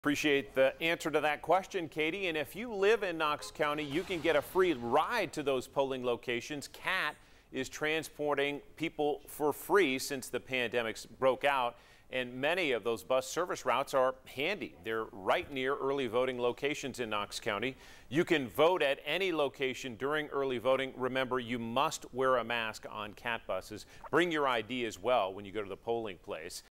KAT offering free rides to voting locations
You must wear a mask and bring an ID.